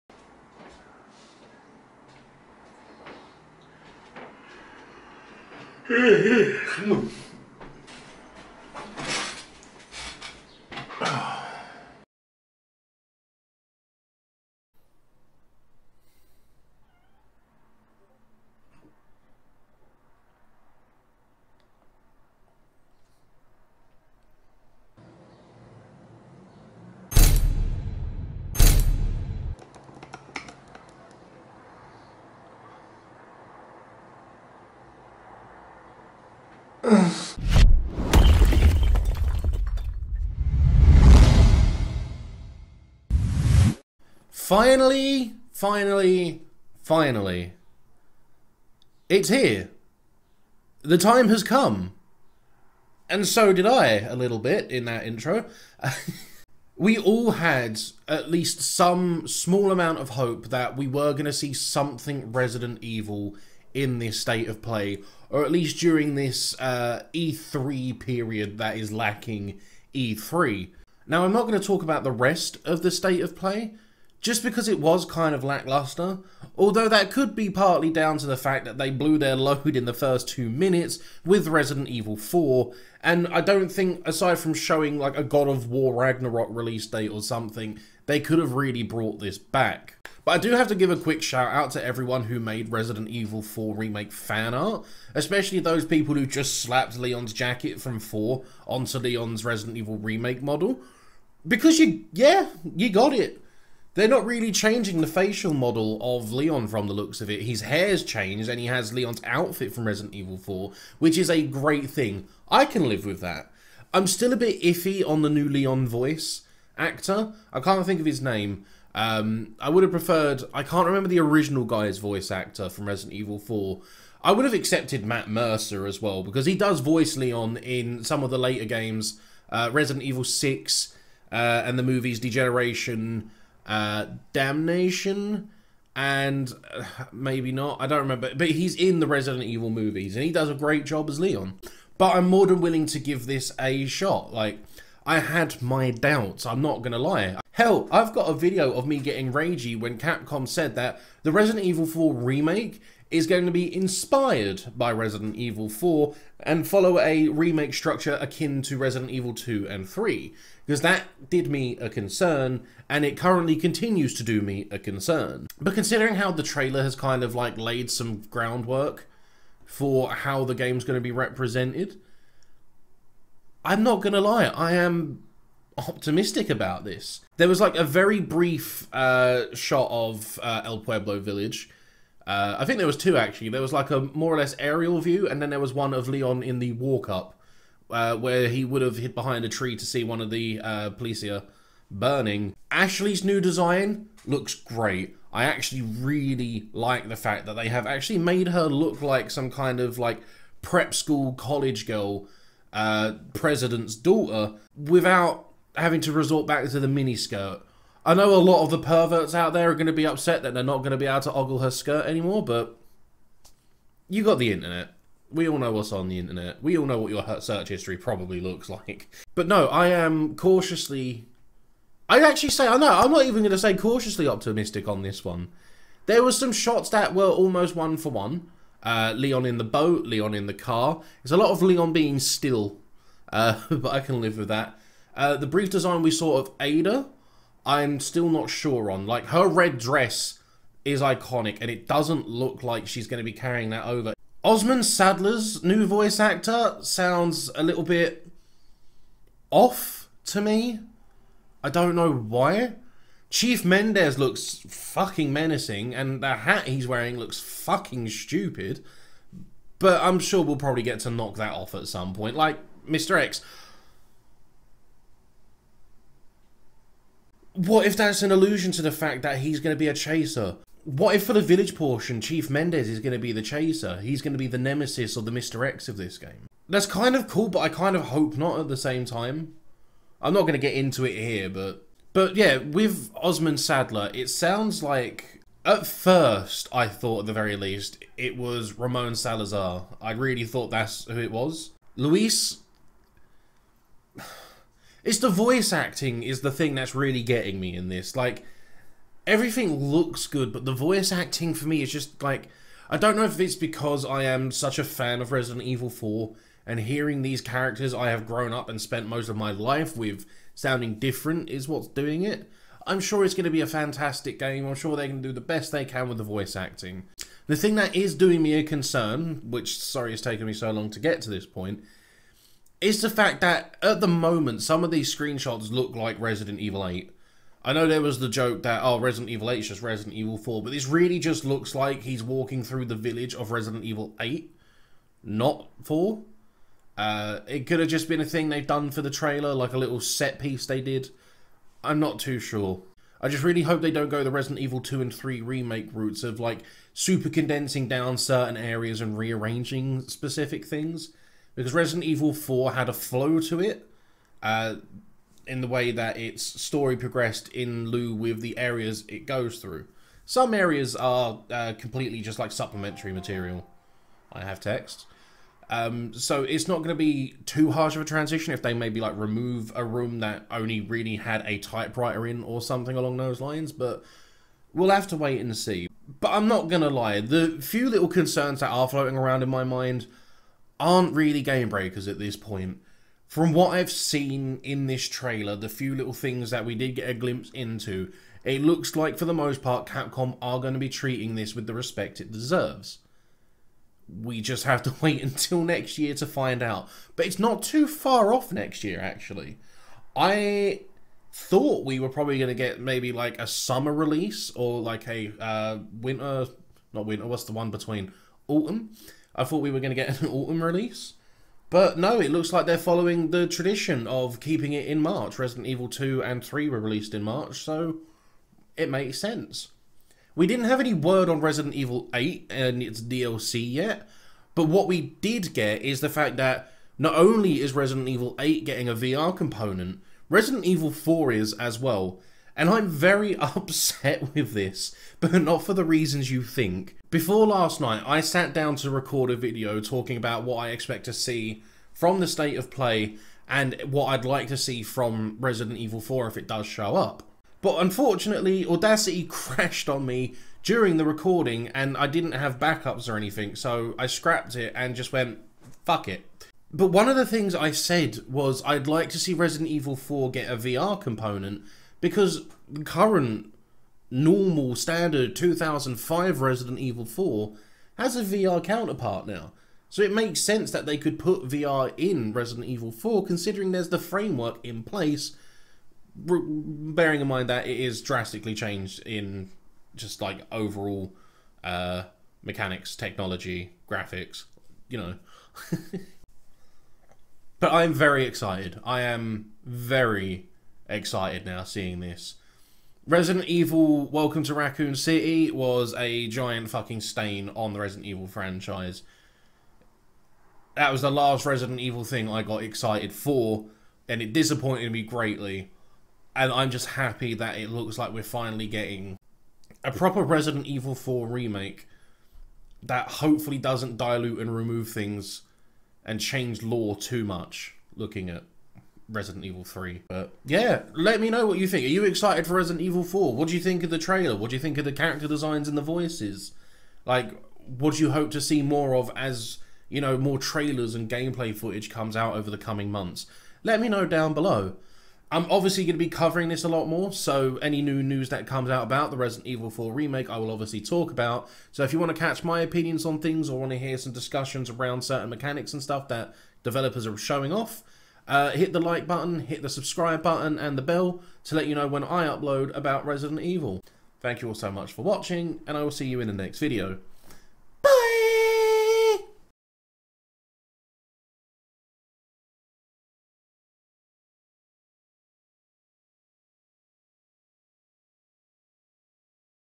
Resident Evil 4 Remake is Officially A Thing!
Capcom came out swinging last night during the State of Play!